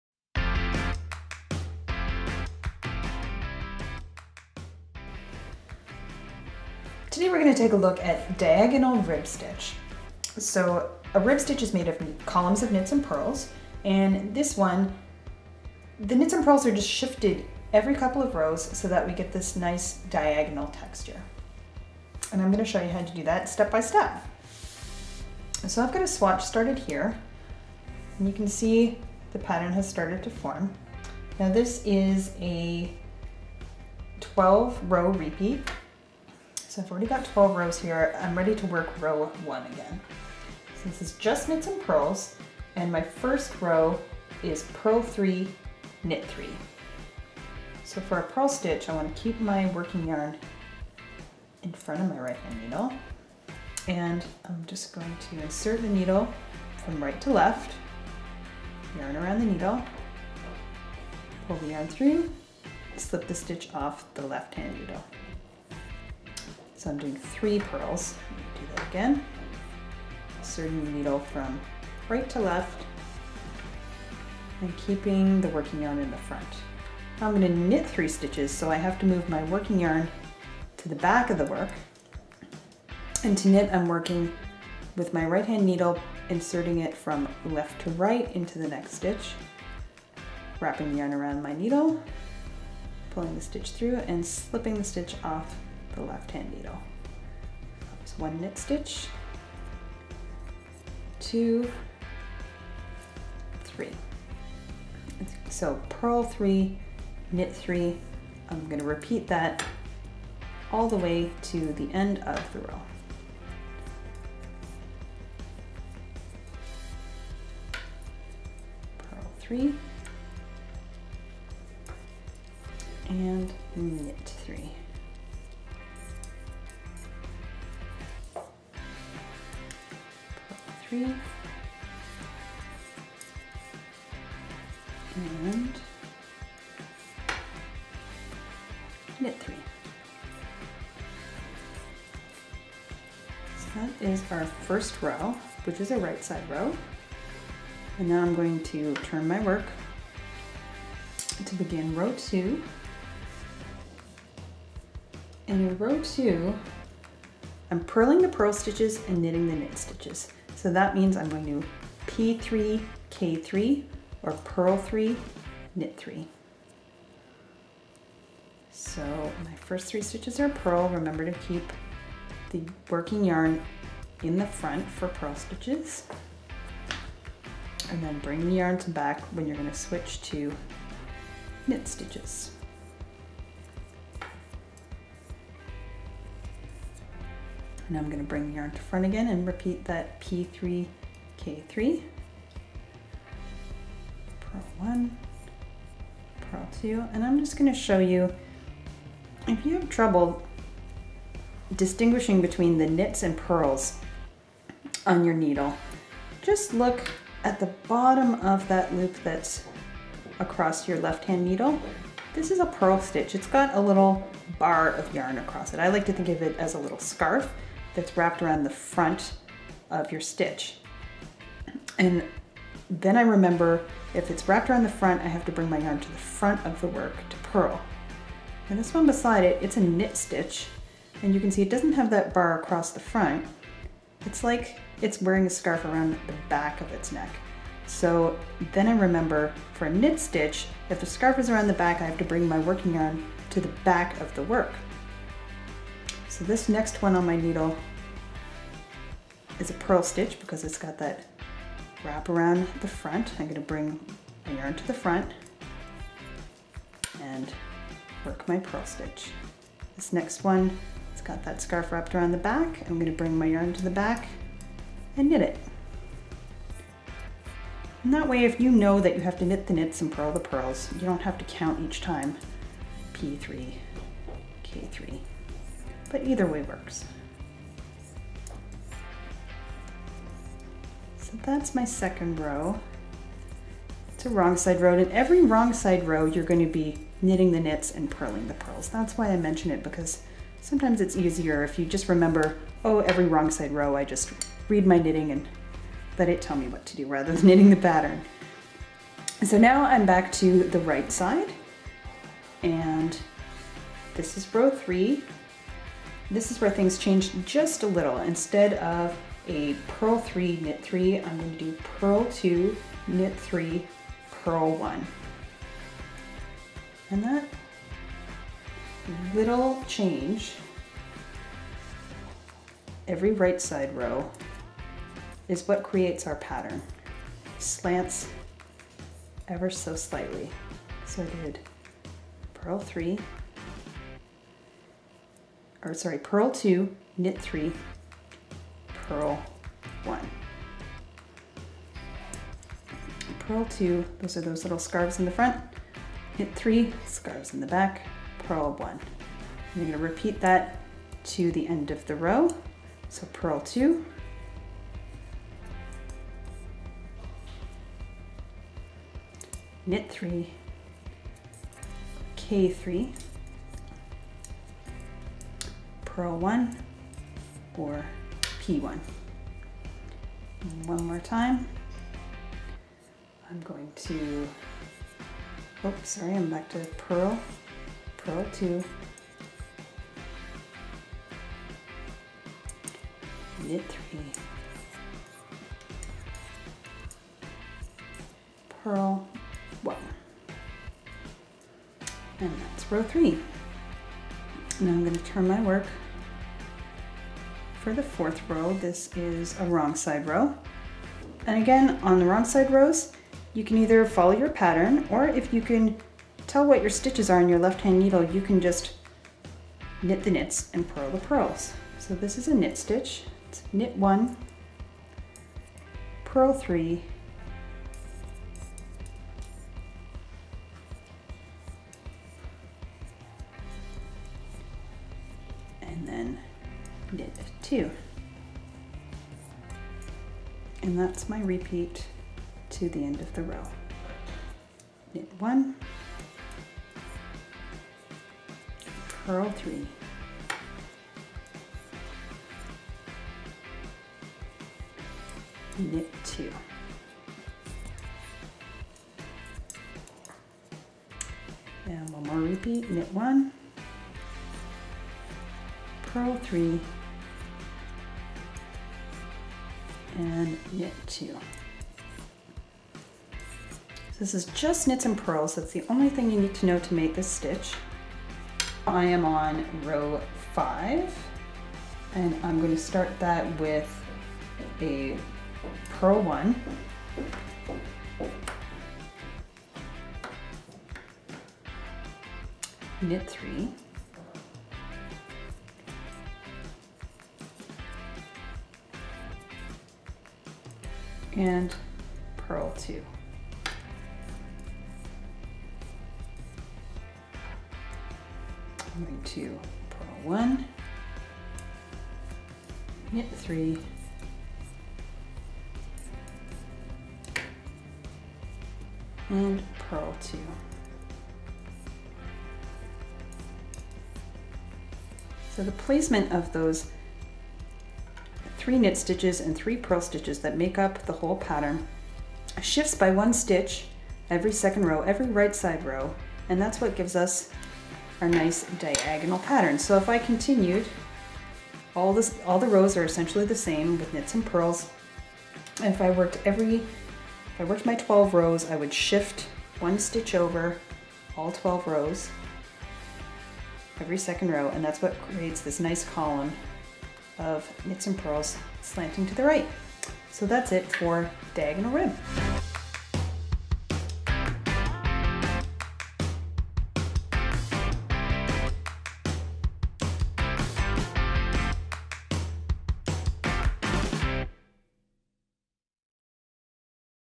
How-To Knit the Diagonal Rib Stitch
Learn how to knit the Diagonal Rib Stitch, used in the Caron United Mystery Adventure Scarf KAL/CAL.